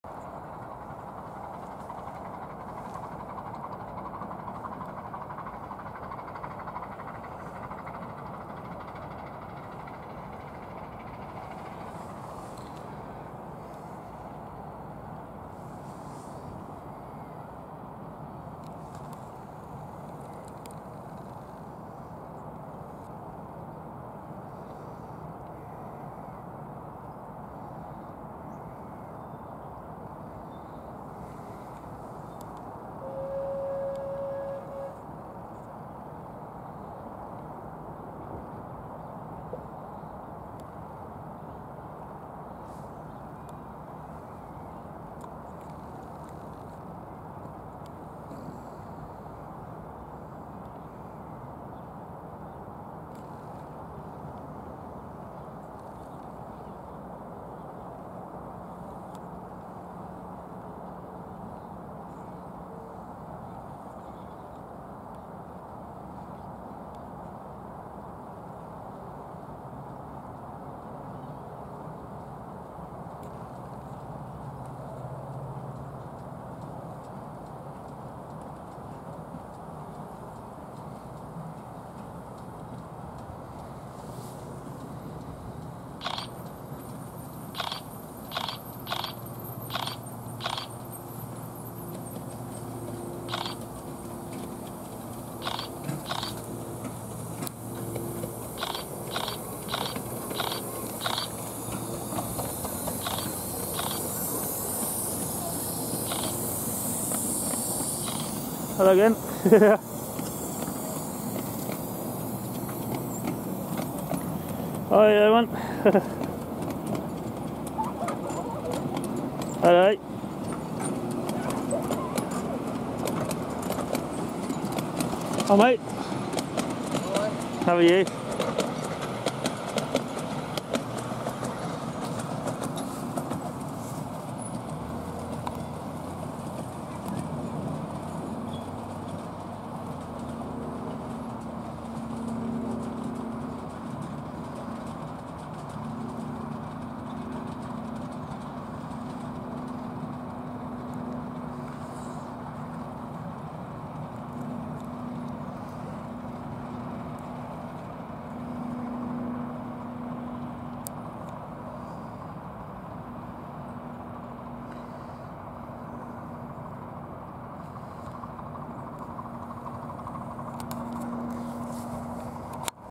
Eastleigh Lakeside Railway No 1908 on The Santa Specials
I went to Eastleigh Lakeside Railway on Tuesday 20th December 2022. Do some landside as the Steam train was doing some runs on The Santa Specials. And Loco was in steam to today was No.1908 Ernest Henry Upton. As this Loco was home fleet at Eastleigh Lakeside Railway. And last time I went here was in 2021. And this one was steaming well in weather.